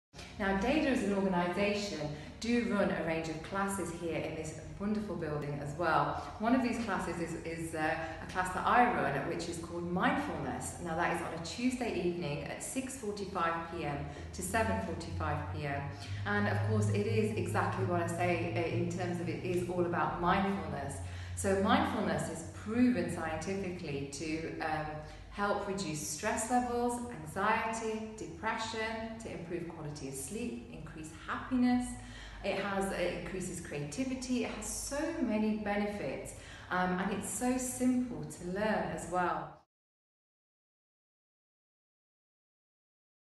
Classes | Mindfulness
Déda's Mindfulness tutor and board member, Pam Sidu discusses her weekly Mindfulness class in the excerpt taken from Déda's World Health Day campaign in 2018. Transcription below.

"Now Déda as an organisation do run a range of classes here in this wonderful building as well. One of these classes is a class that I run which is called Mindfulness. That is on a Tuesday evening at 6.45pm - 7.45pm and it is of course exactly what I say, in terms of it is all about Mindfulness. So Mindfulness is proven scientificially to help reduce stress levels, anxiety, depression, to improve quality of sleep, increase happiness, increases creativity. It has so many benefits and it is so simple to learn as well!"